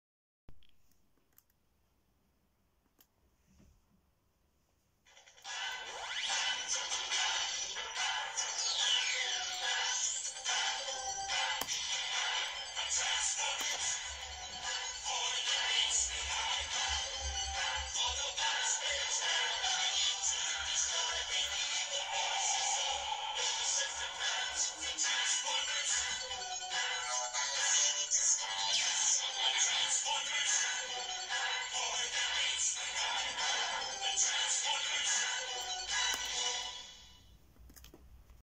David/Thomas PowerPoint Presentation: Transformers Adventures on Cybertron Opening Title
This is the intro for my Seperate series
Transformers Adventures on Cybertron

Transformers is owned by Hasbro